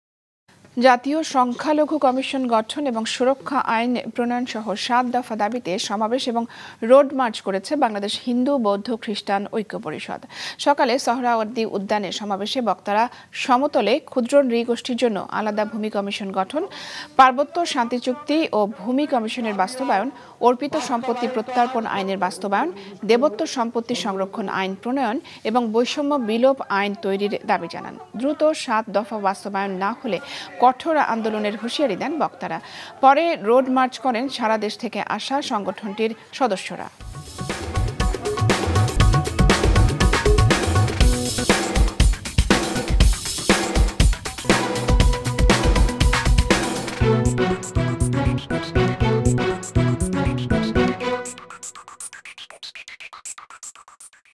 জাতীয় সংখ্যালঘু কমিশন গঠন ও সুরক্ষা আইন প্রণয়ণসহ সাত দফা দাবি হিন্দু বৌদ্ধ খ্রিস্টান ঐক্য পরিষদ।
2 Bir Uttam Ziaur Rahman Road (New Airport Road)